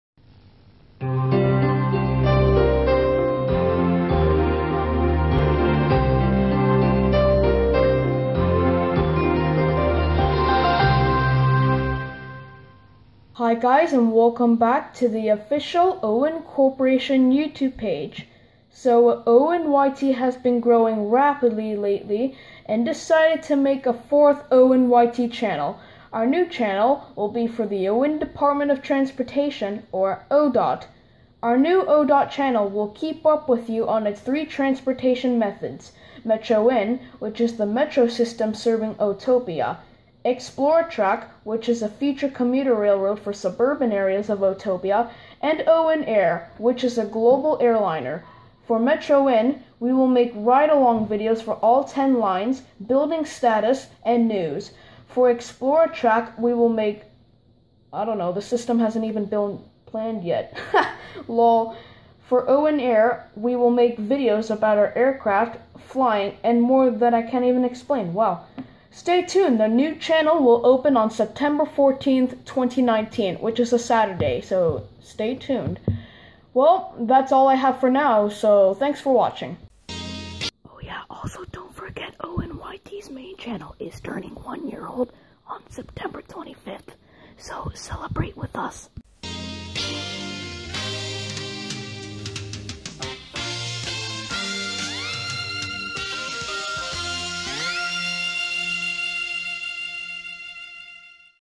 New Channel Announcement! | Polara YT
The O-INN Department of Transportation (O-DOT) is planning to have its own channel, which will keep up with you on its transportation network!

Planned release date: September 14, 2019